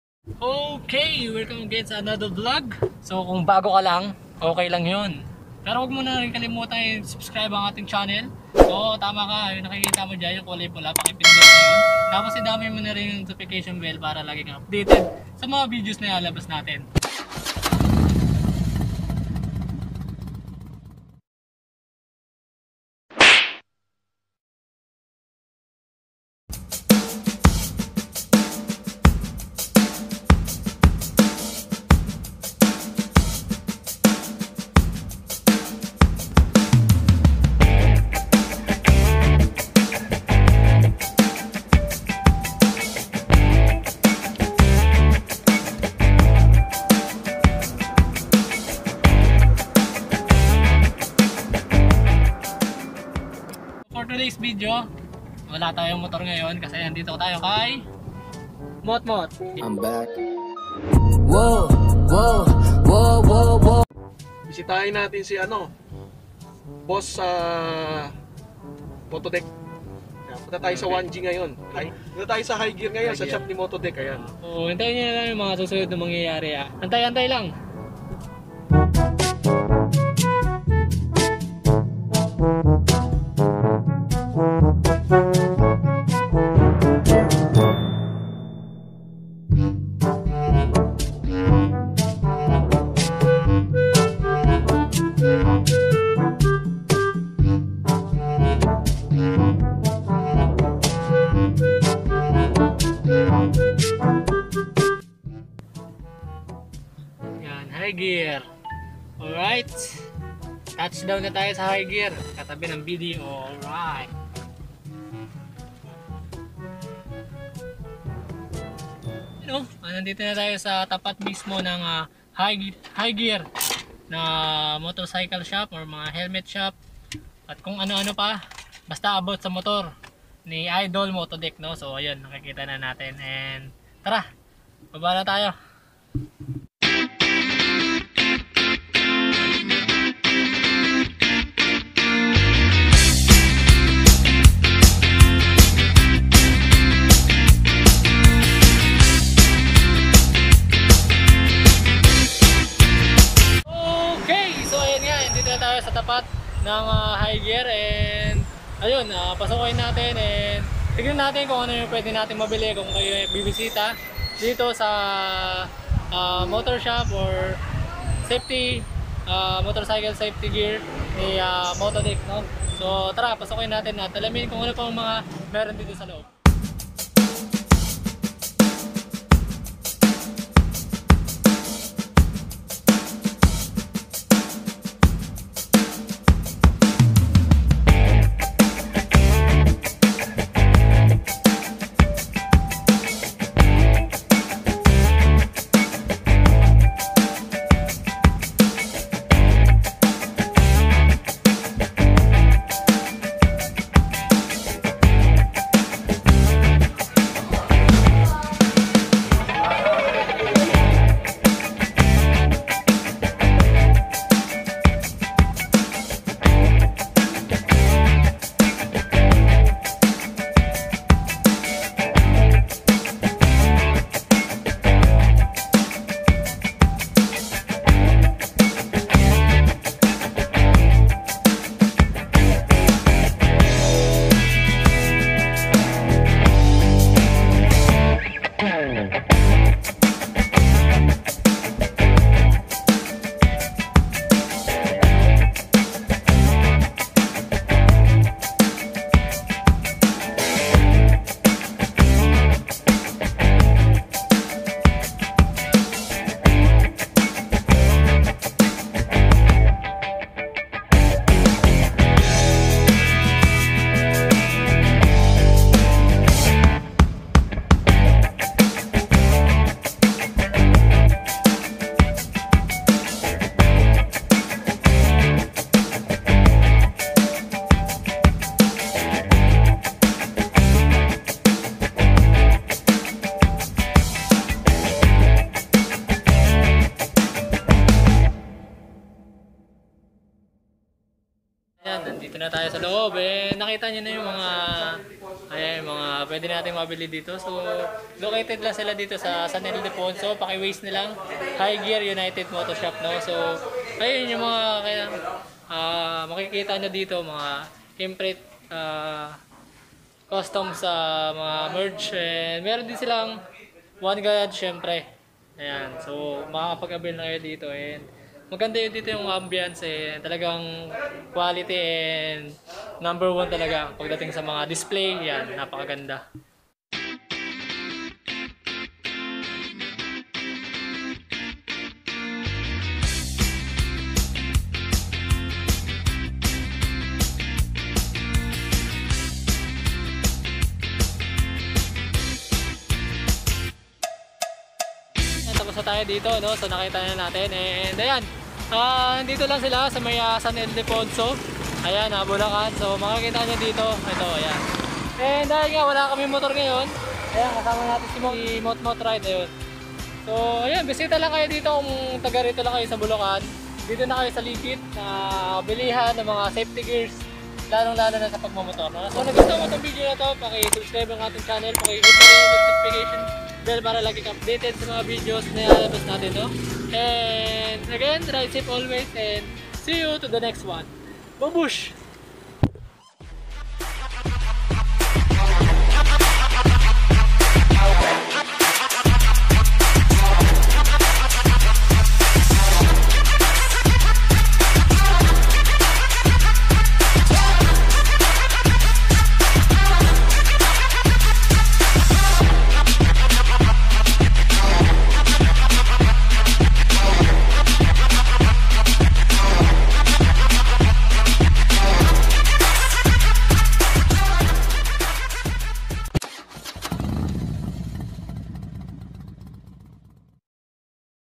HIGH GEAR UNITED MOTOSHOP NI TRUEPA | MOTODECK | WORLD CLASS SHOP | MARPSMOTOPH
Tara na at puntahan ang mala-world class na motoshop ni truepa motodeck .

Maraming mga available and affordable safety gears ang narito.